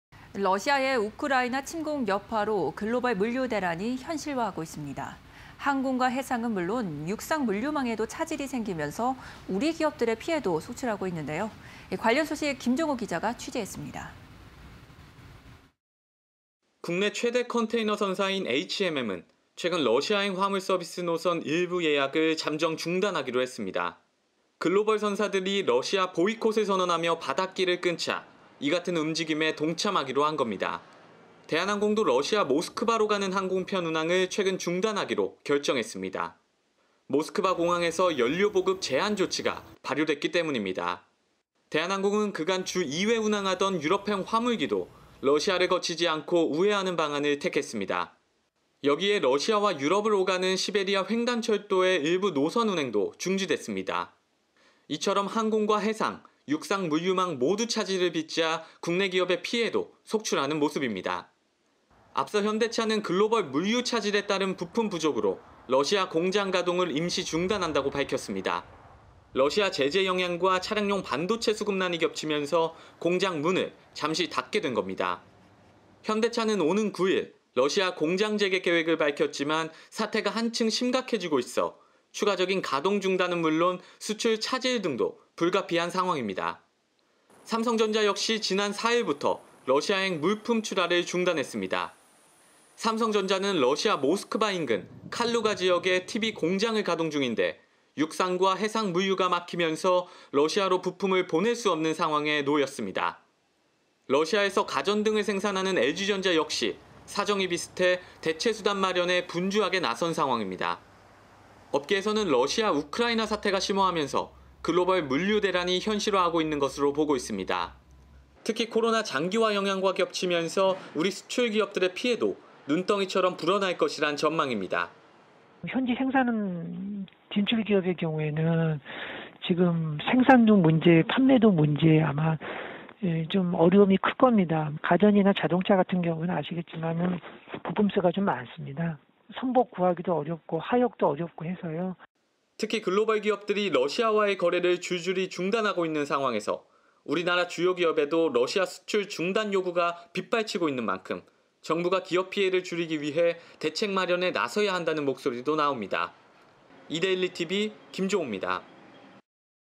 러시아發 물류대란 현실로... 현대차·삼성전자 등 피해 속출 (20220307)
- HMM, 러시아행 화물 노선 일부 예약 잠정 중단
- 대한항공, 러시아 항공편 운항 중단... 화물기도 우회키로
- 현대차, 러시아 공장 가동 임시 중단... 생산·수출 차질
- 삼성전자, 러시아행 물품 출하 중단... 대책 마련 분주